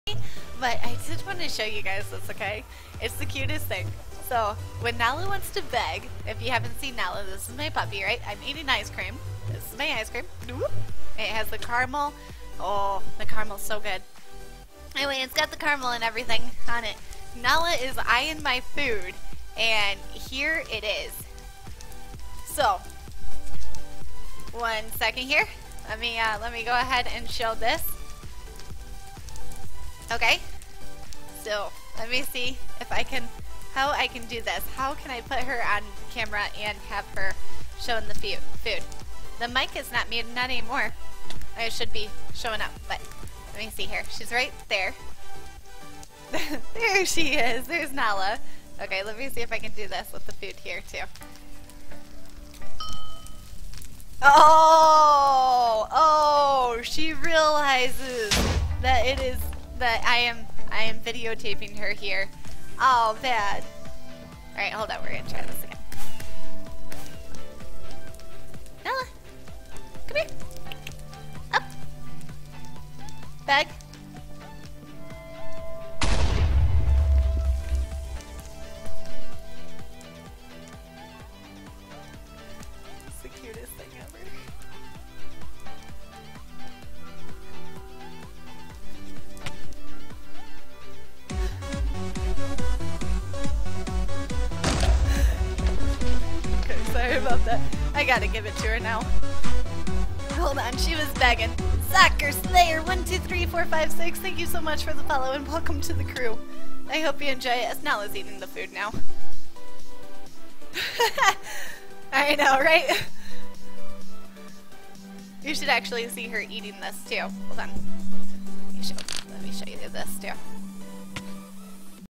NALA "Give Me Food Please Momma"
I show Nala doing her cute little beg...don't worry she gets ice cream for it =D -- Watch live